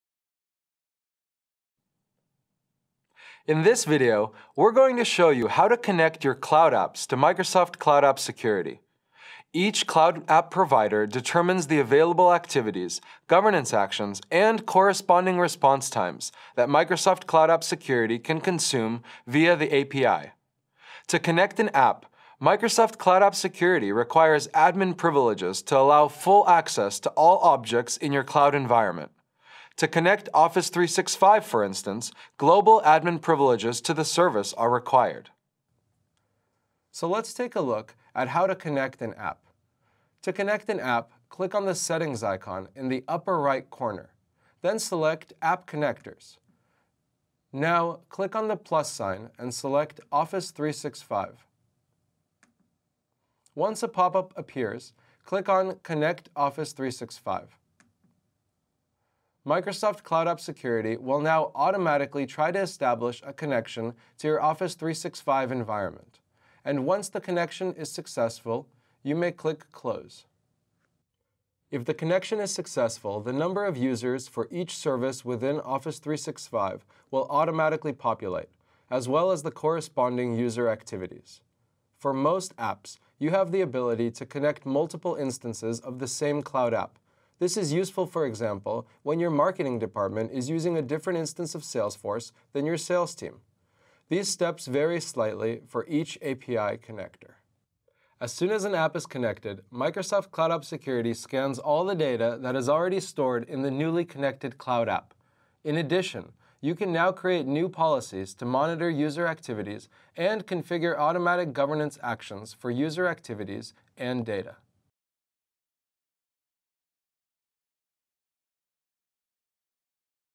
Connecting apps to Microsoft Cloud App Security
In this video we show you how to connect Microsoft and non-Microsoft applications to Microsoft Cloud App Security and enable our powerful capabilities across DLP, Threat Protection and more.